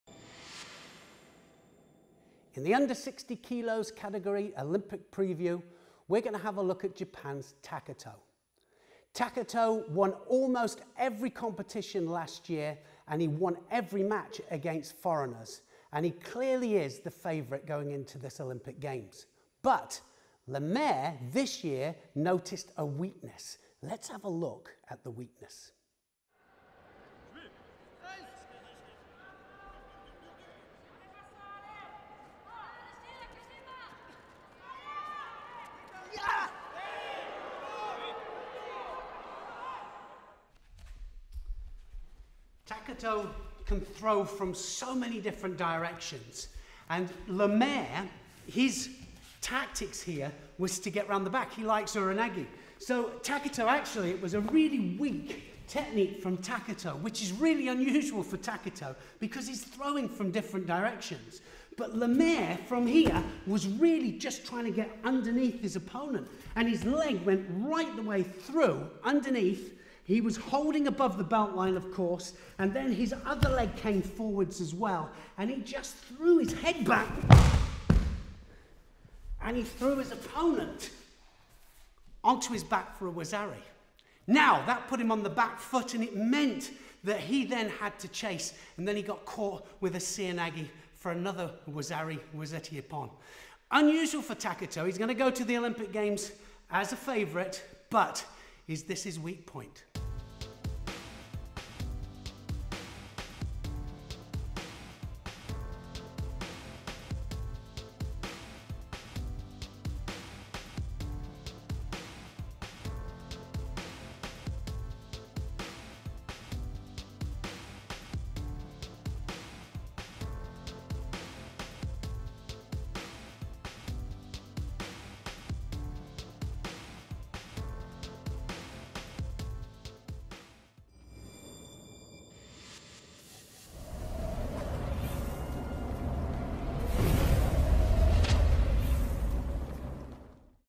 -60kg Category Preview
Former World Champion Neil Adams​ focuses on Japan's 2013 World Champion Naohisa Takato in the -60kg!